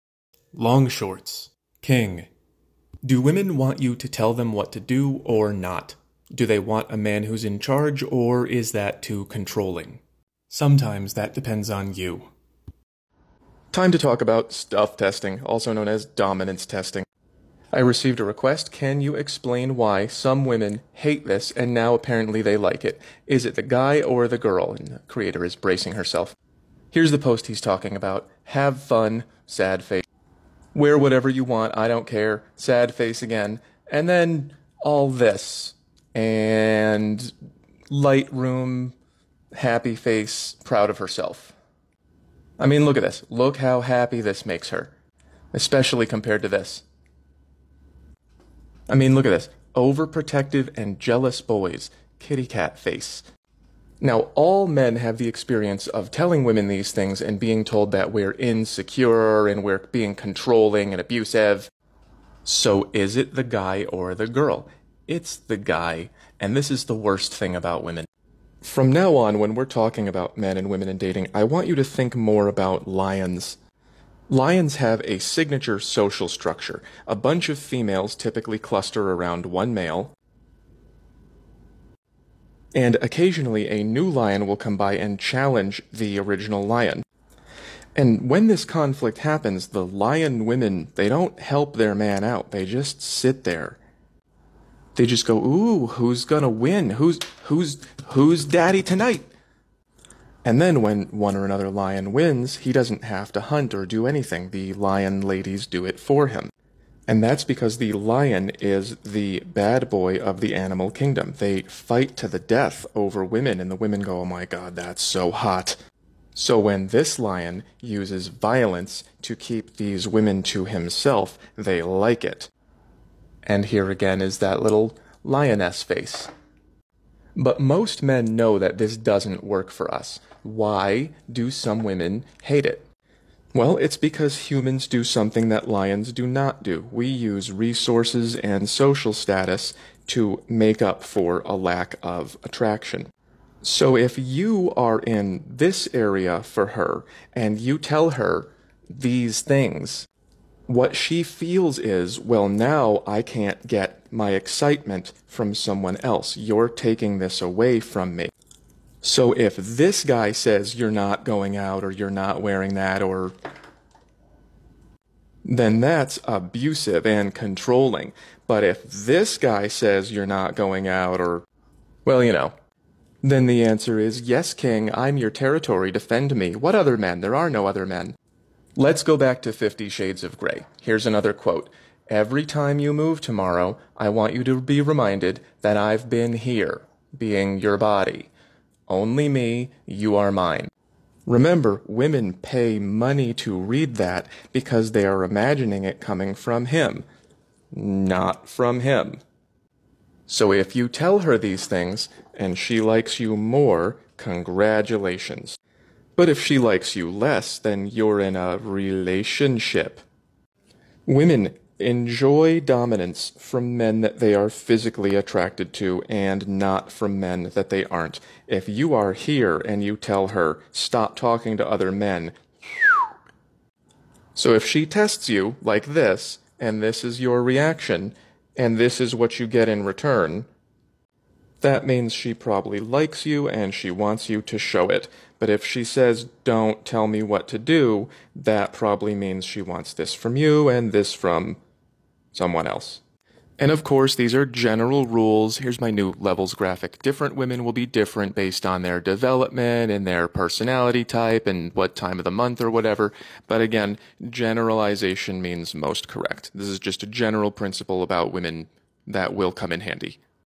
King
Why do women sometimes hate it when you're possessive, but sometimes love it?